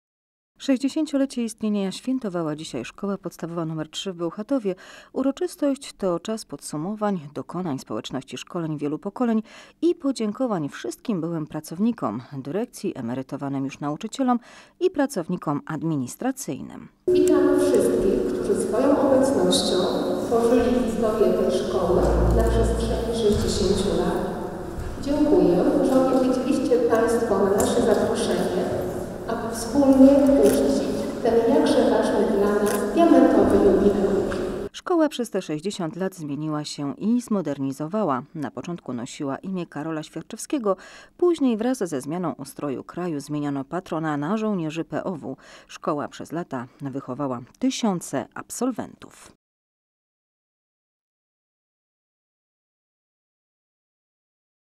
TKB - Jubileusz SP 3 - 23.11.2018
60-lecie istnienia świętowała dzisiaj Szkoła Podstawowa nr 3 w Bełchatowie. Uroczystość to czas podsumowań dokonań społeczności szkolnej wielu pokoleń i podziękowań wszystkim byłym pracownikom, dyrekcji, emerytowanym już nauczycielom i pracownikom administracyjnym.

Szkoła przez te 60 lat zmieniała się i modernizowała. Na początku nosiła imię Karola Świerczewskiego, później wraz ze zmianą ustroju kraju zmieniono patrona na Żołnierzy POW. Szkoła przez lata wychowała tysiące absolwentów.